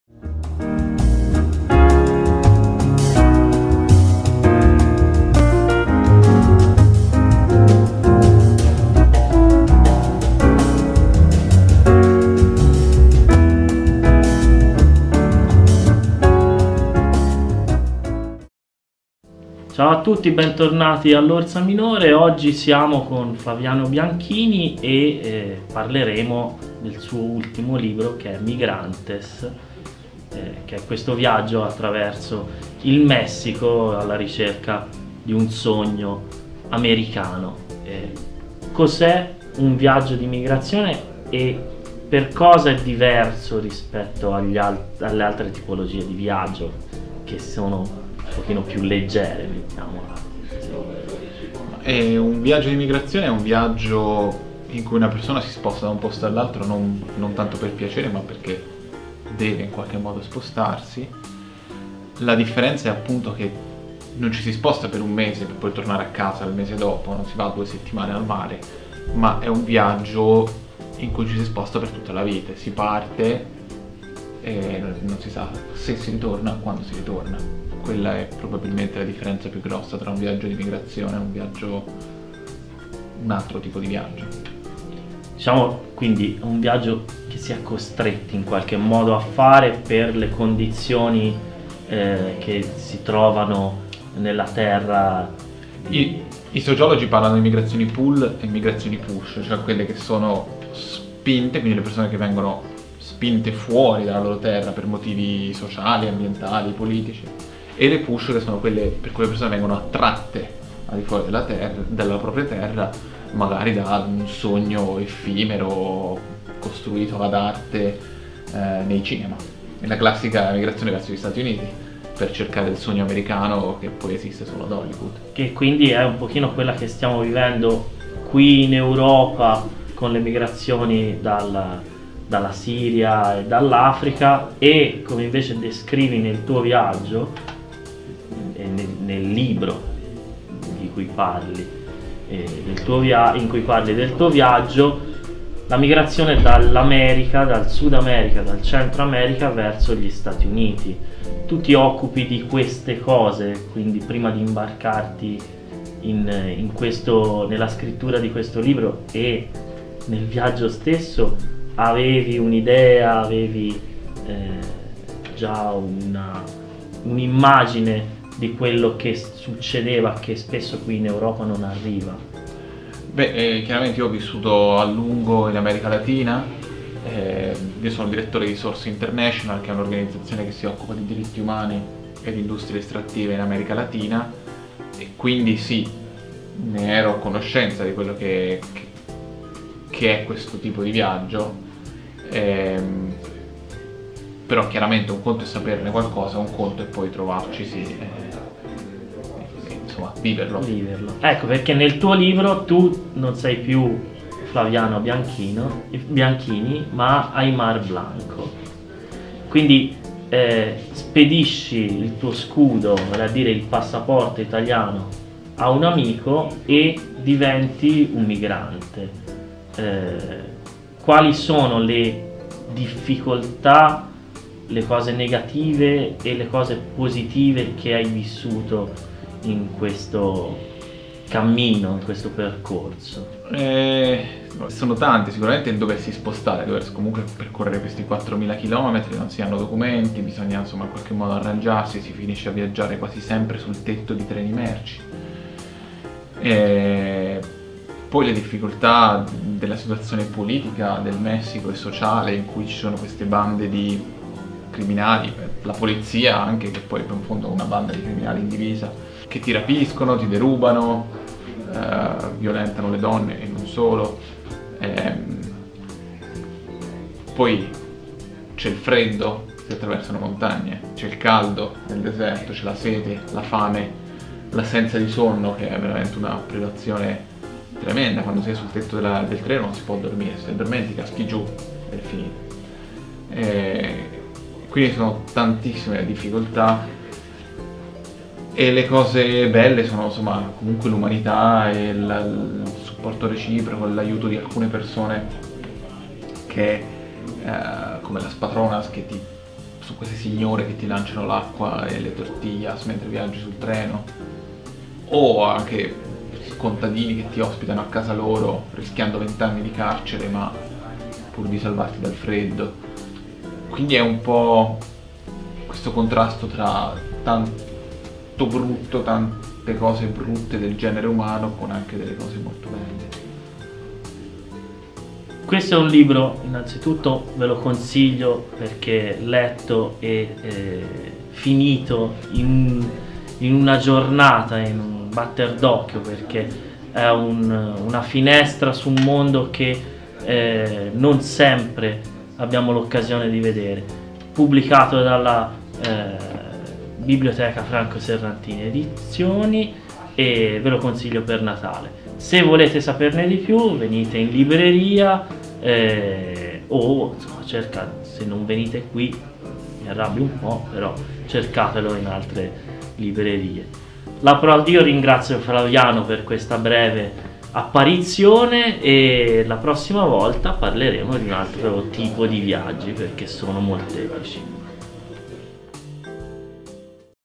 Pisa in TV - Letteratura di viaggio - Flaviano Bianchini presenta "Migrantes"
Migrantes, l’incredibile storia di Flaviano e di un viaggio clandestino verso il sogno americano, raccontata alla libreria l'orsa minore.